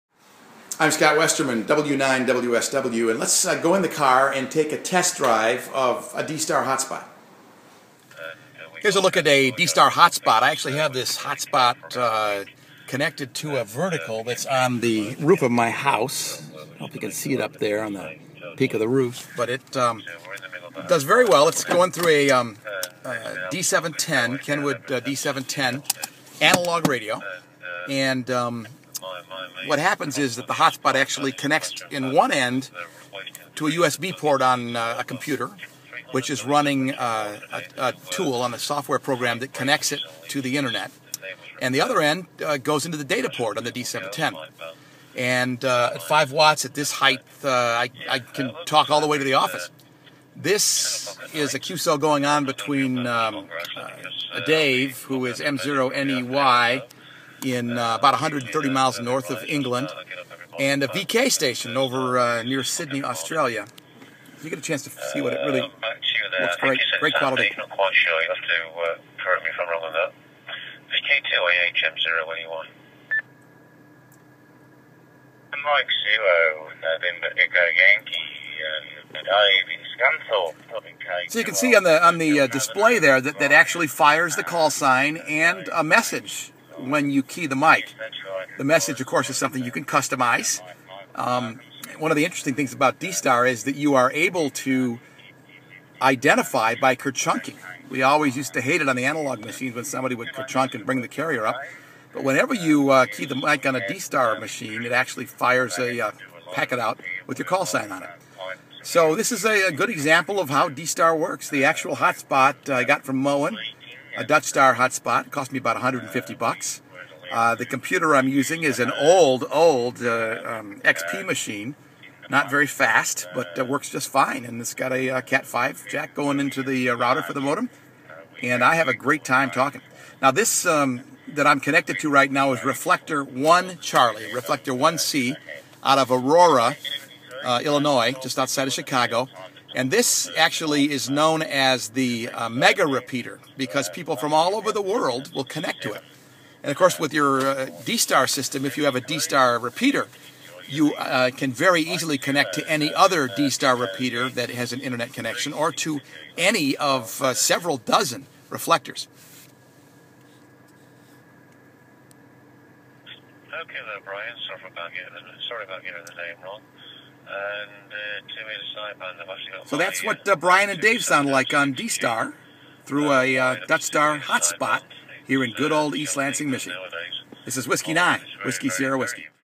D-Star Hotspot Demo
W9WSW Demos the DutchStar D-Star Hotspot